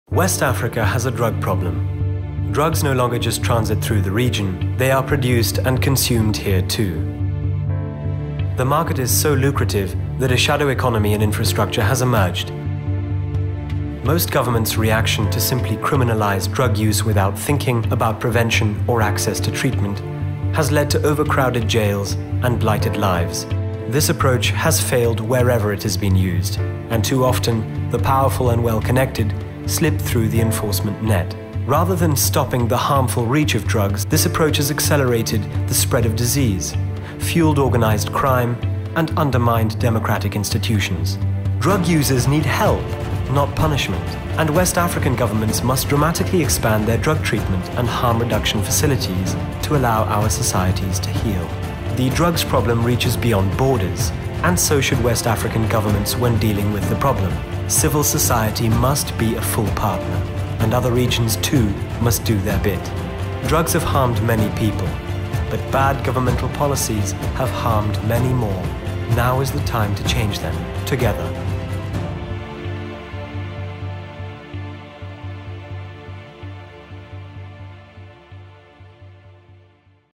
West Africa has a Drug Problem - Animation
A destructive threat is jeopardizing progress in West Africa and beyond: with local collusion, international drug cartels are undermining our countries and communities, and devastating lives.

After looking at the evidence, consulting experts from the region and around the world, and visiting some of the most affected countries and communities in West Africa, the West Africa Commission on Drugs makes bold recommendations tackling this issue.